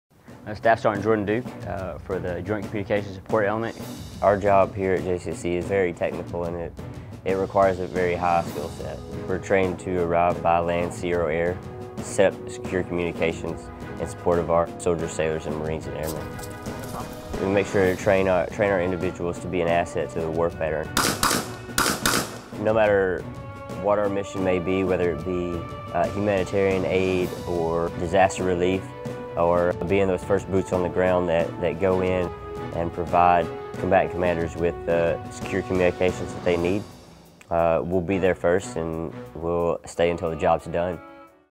MacDill's JCSE
The Joint Communication's Support Element operates in any environment. Experts like Staff Sergeant Jordan Duke ensure that combatant commanders have the communications essential to win the battle.

Releasing Authority:
Capt Kathleen Ice USAF
6 AMW/PA MacDill AFB